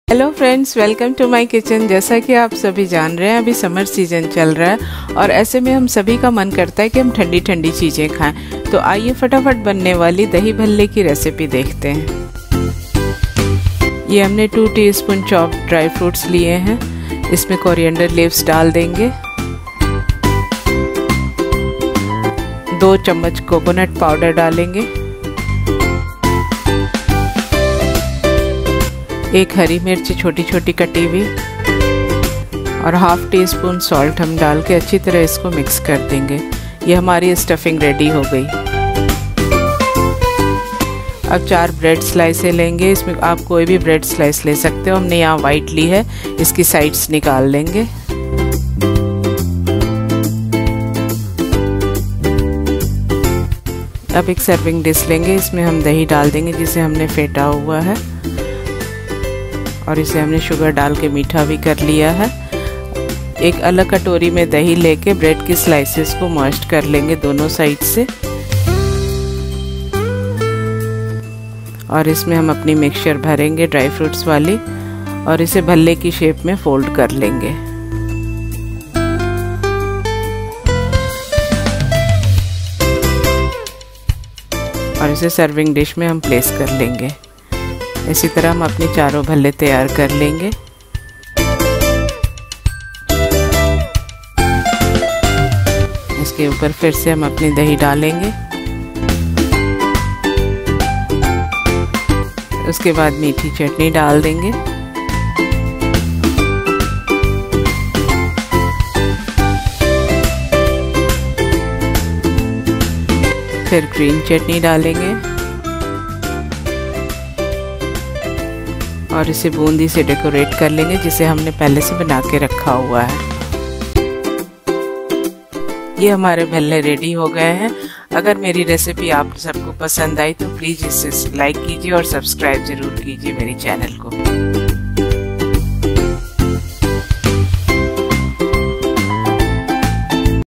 How to make Instant and Creamy Dahi Bhalla|Without Frying | Super Soft and Delicious|Geeta's Kitchen
Ingredients --
*Dry Fruits(Cashew,Almond,Raisins and Pistachios) - 2 tsp
*Coconut Powder - 2 tsp
*Coriander Leaves
*Dahi(Beaten with sugar,salt and cumin powder) -  2 cup
*Bread Slices
*Chutneys for garnishing
*Boondi for garnishing